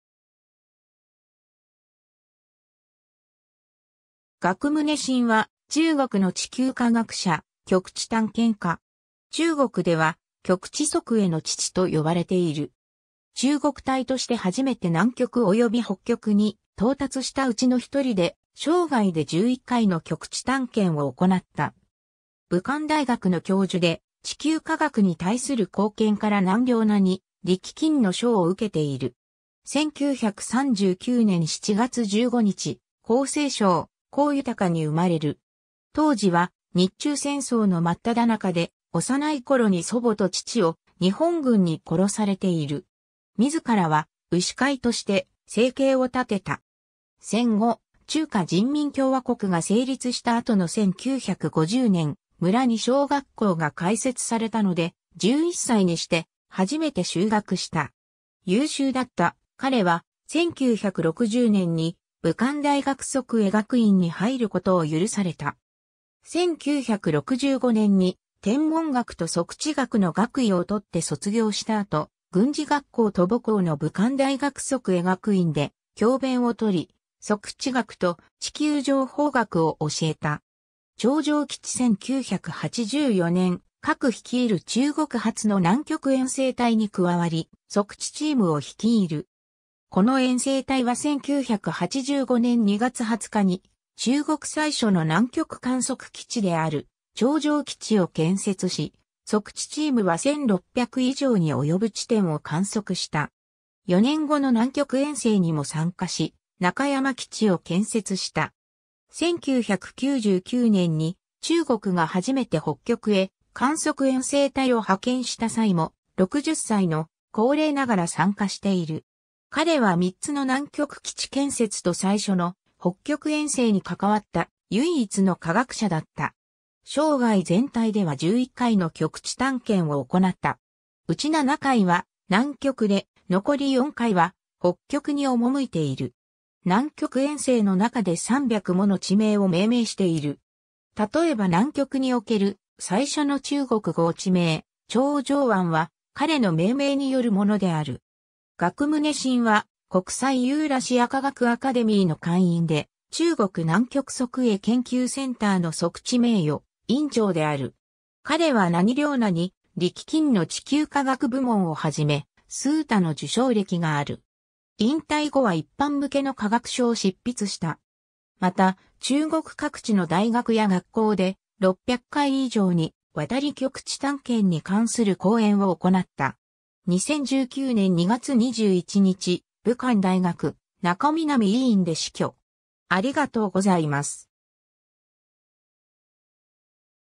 鄂棟臣
鄂棟臣 (簡体字: 鄂栋臣; 1939年7月15日 – 2019年2月21日) は、中国の地球科学者、極地探検家。
中国では「極致測絵（測量と製図）の父」と呼ばれている。
中国隊としてはじめて南極および北極に到達したうちの一人で、生涯で11回の極地探検を行った。
武漢大学の教授で、地球科学に対する貢献から何梁何利基金の賞を受けている。
 1939年7月15日、江西省広豊に生まれる。
当時は日中戦争の真っただ中で、幼いころに祖母と父を日本軍に殺されている。
自らは牛飼いとして生計を立てた。
 戦後、中華人民共和国が成立した後の1950年、村に小学校が開設されたので、11歳にして初めて就学した。
優秀だった彼は、1960年に武漢大学測絵学院に入ることを許された。
1965年に天文学と測地学の学位を取って卒業した後、軍事学校と母校の武漢大学測絵学院（後に武漢大学に統合）で教鞭をとり、測地学と地球情報学を教えた。
 長城基地（2011年） 1984年、郭琨率いる中国初の南極遠征隊に加わり、測地チームを率いる。
この遠征隊は1985年2月20日に中国最初の南極観測基地である長城基地を建設し、測地チームは1600以上に及ぶ地点を観測した。
4年後の南極遠征にも参加し、中山基地を建設した。
 1999年に中国が初めて北極へ観測遠征隊を派遣した際も、60歳の高齢ながら参加している。
彼は3つの南極基地建設と最初の北極遠征に関わった唯一の科学者だった。
生涯全体では11回の極地探検を行った。
うち7回は南極で、残り4回は北極に赴いている。
南極遠征の中で300もの地名を命名している。
例えば南極における最初の中国語地名「長城湾」は、彼の命名によるものである。
 鄂棟臣は国際ユーラシア科学アカデミーの会員で、中国南極測絵研究センターの測地名誉院長である。
彼は何梁何利基金の地球科学部門をはじめ、数多の受賞歴がある。
 引退後は一般向けの科学書を執筆した。
また中国各地の大学や学校で、600回以上にわたり極地探検に関する講演を行った。
 2019年2月21日、武漢大学中南医院で死去。